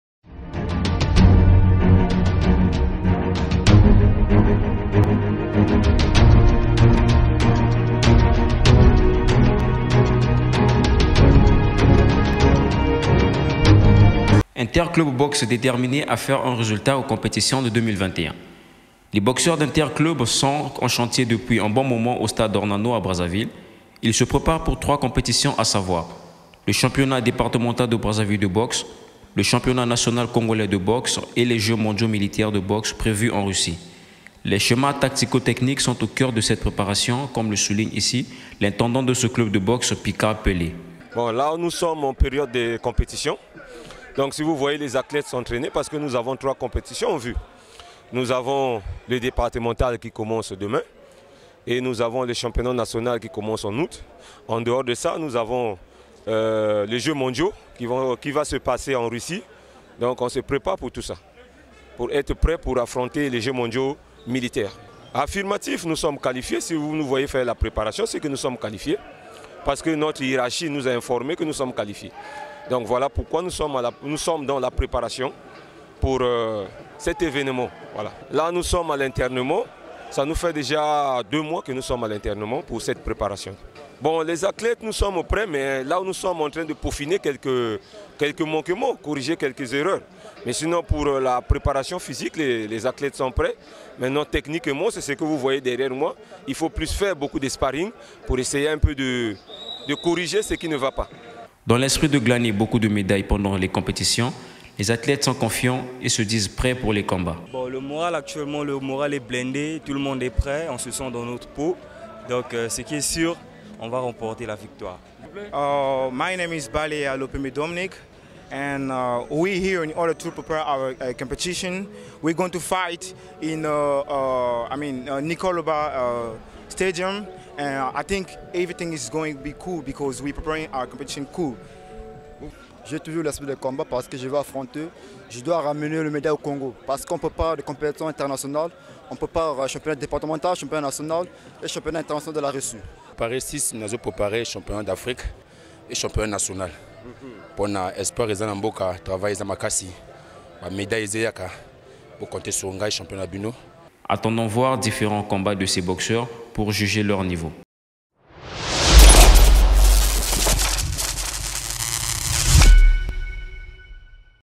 Préparation des boxeurs d'Inter Club
Les boxeurs d'Inter Club sont en chantier depuis un bon moment où ils peaufinent les stratégies pour 3 compétitions.